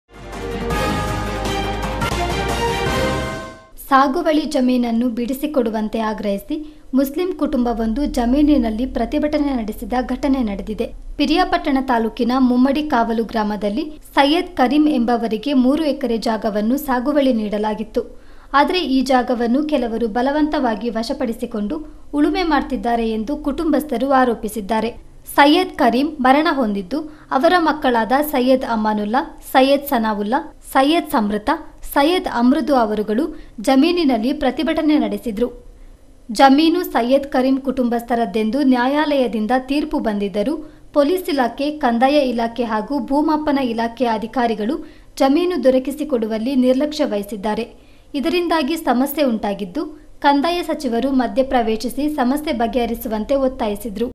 Give up our fields; Muslim family protest ‘ಸಾಗುವಳಿ ಜಮೀನನ್ನು ಬಿಡಿಸಿಕೊಡಿ’ ; ಮುಸ್ಲಿಂ ಕುಟುಂಬ ಪ್ರತಿಭಟನೆ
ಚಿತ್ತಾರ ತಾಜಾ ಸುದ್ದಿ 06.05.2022